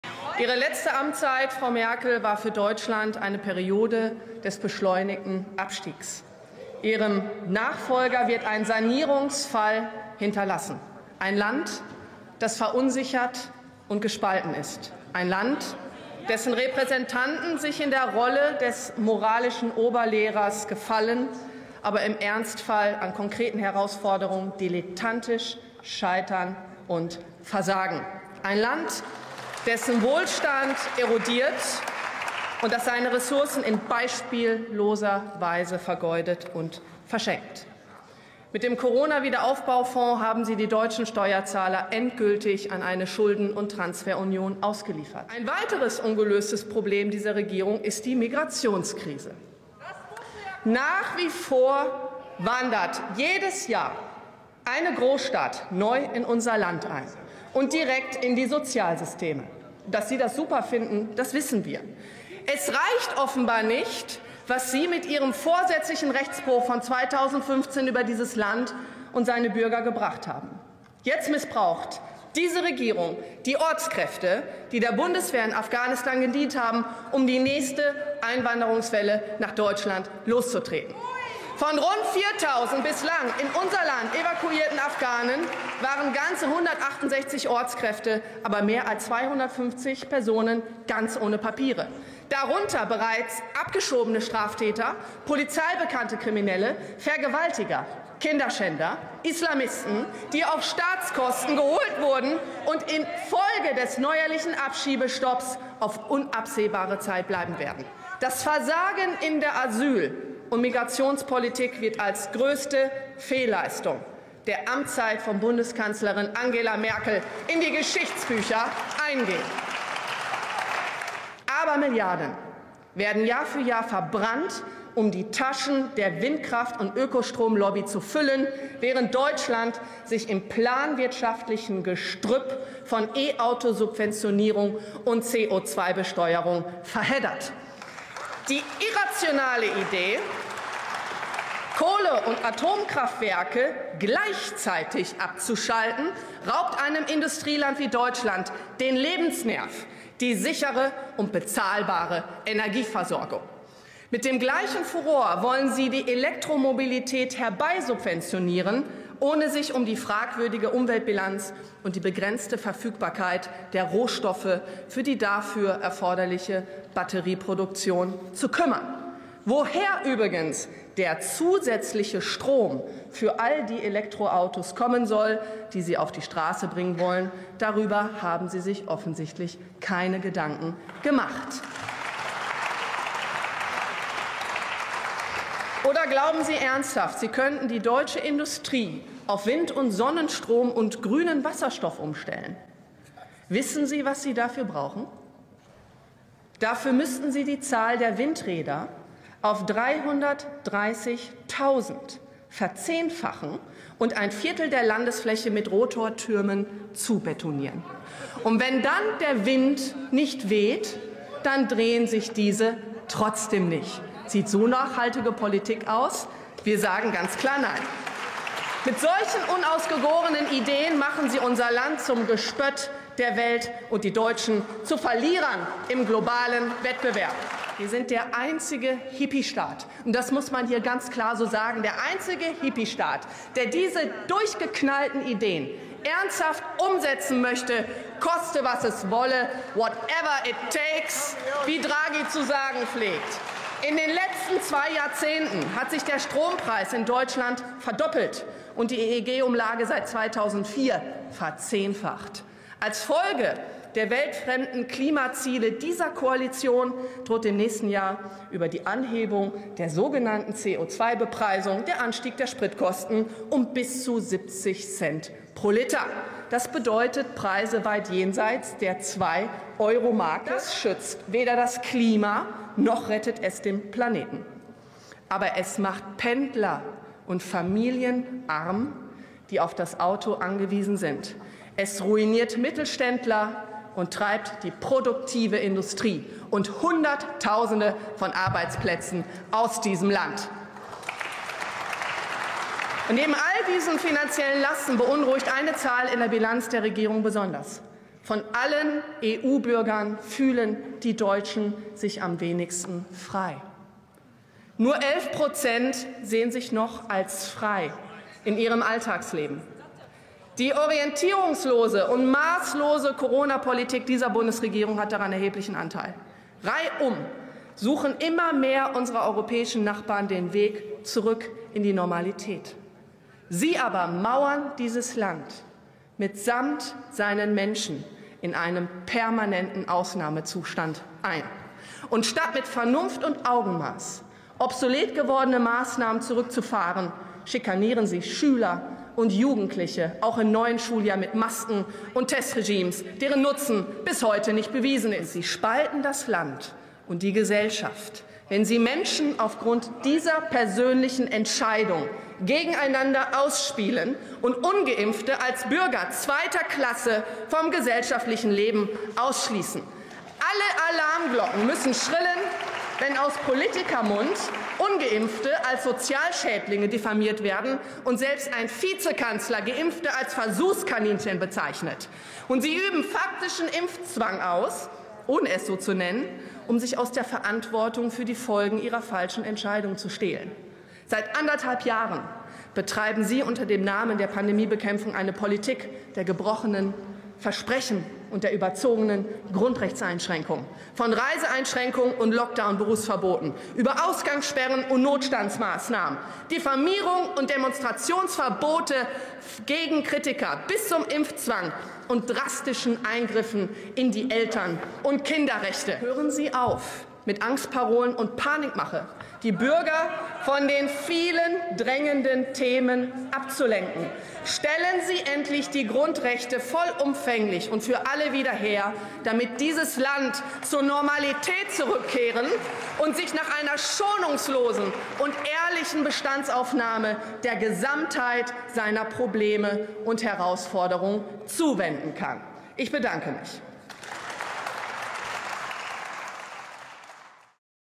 7.9.2021 Grandiose Abrechnung mit dem System MERKEL. Chapeau, Frau Dr.Weidel, für diese Rede!!!
Grandiose Rede von Frau Dr.Weidel im Bundestag! Sie rechnet konsequent mit dem Versagen einer Kanzlerin Merkel und dessen Regierung ab. Eine Sternstunde der politischen Rede. Leider ist Deutschland schon zu verstrahlt, um das überhaupt im Ansatz zu erkennen und sein Wahlverhalten danach auszurichten.

LEIDER!

Hier die gesamte Rede (12:50 min)